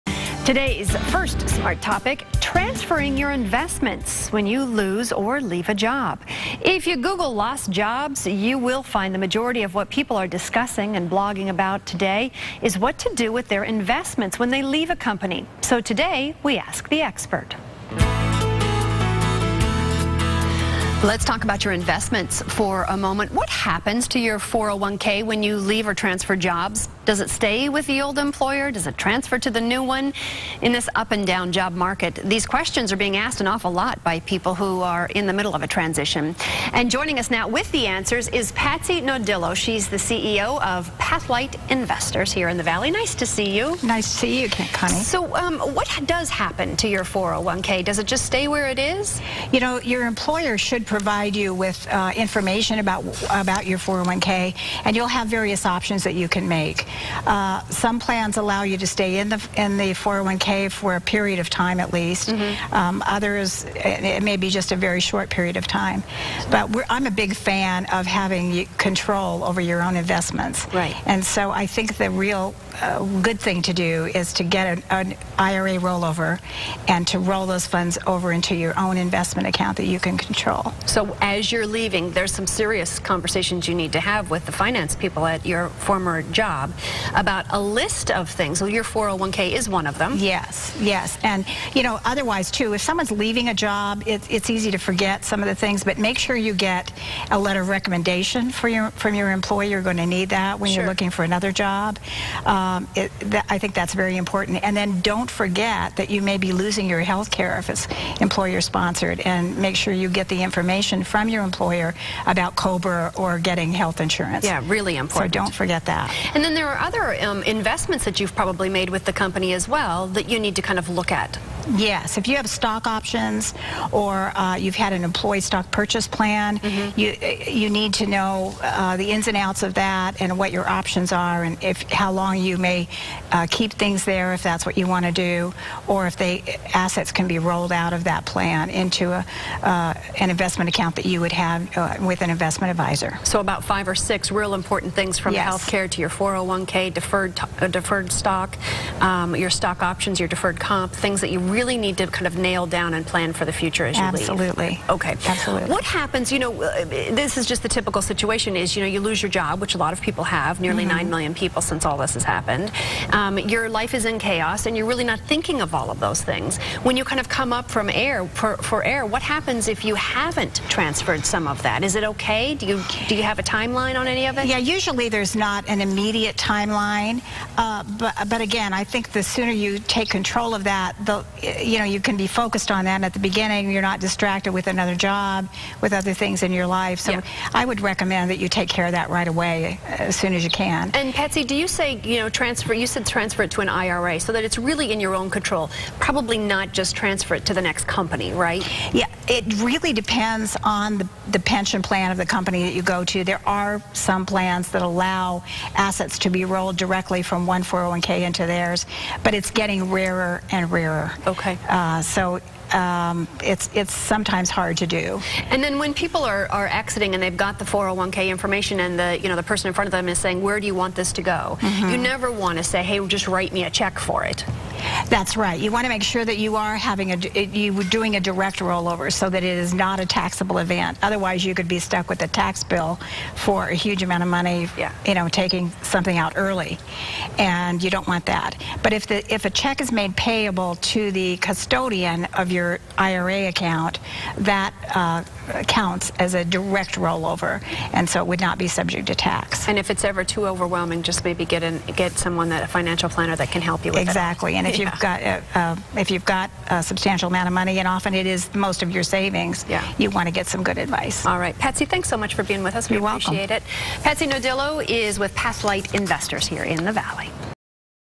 Job loss? Smart Family has some investment transfer advice
With an estimated nine million Americans caught up in the job shuffle and loss over the past 5 years, the question "What do I do with my 401K?" has been asked by many.